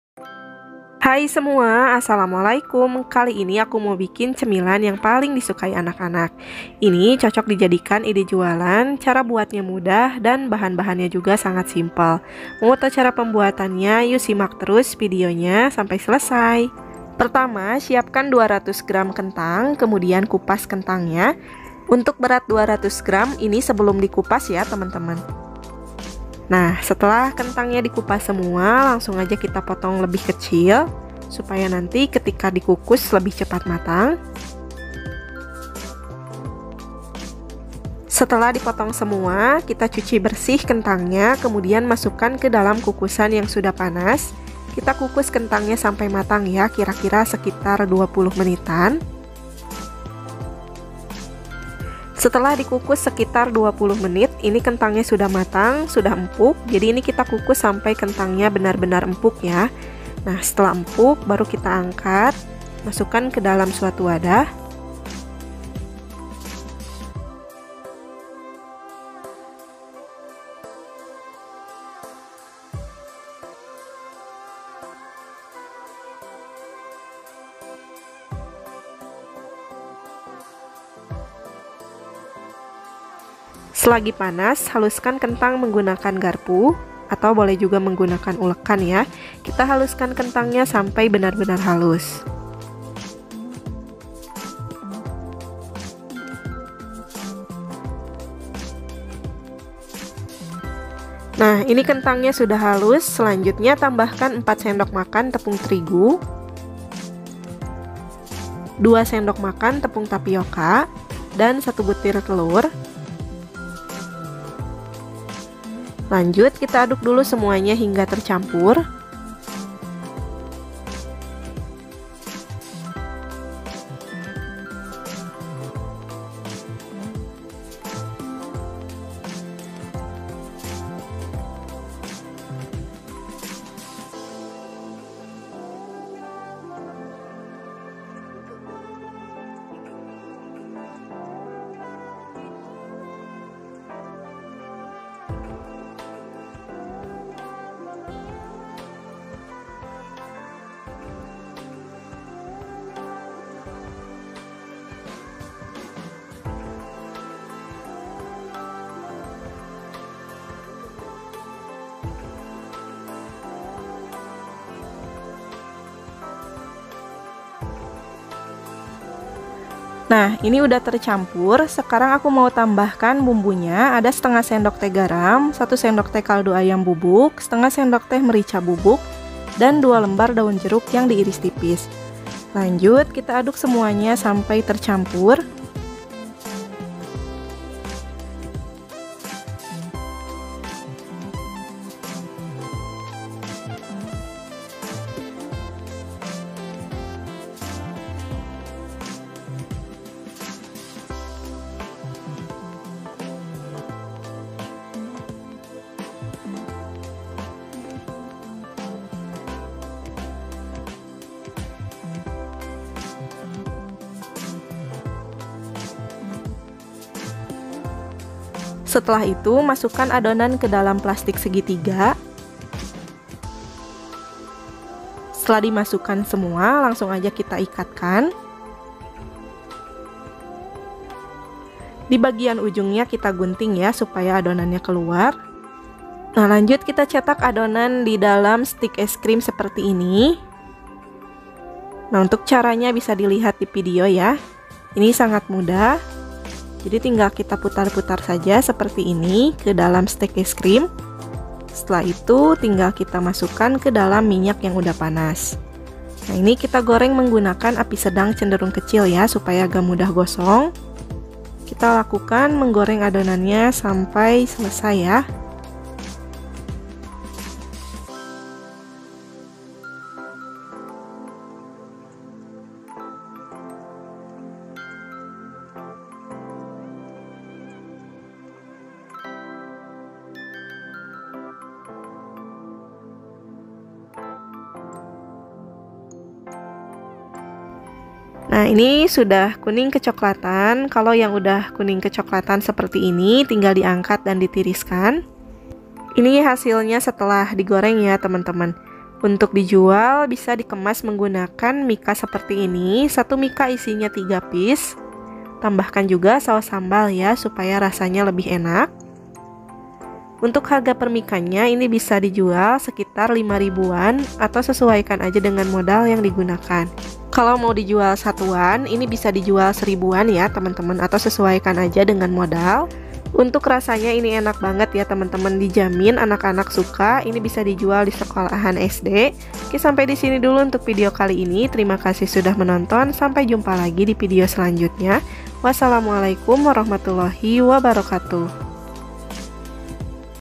Anak anak terkejut dengan resep ini !! Padahal bahannya cuma kentang ga nyangka bakalan selaris ini
Bahan bahan

200 gr kentang
4 sdm terigu
2 sdm tapioka
1 butir telur
1/2 sdt garam
1 sdt kaldu bubuk
1/2 sdt merica bubuk
2 lembar daun jeruk
Stik es krim secukupnya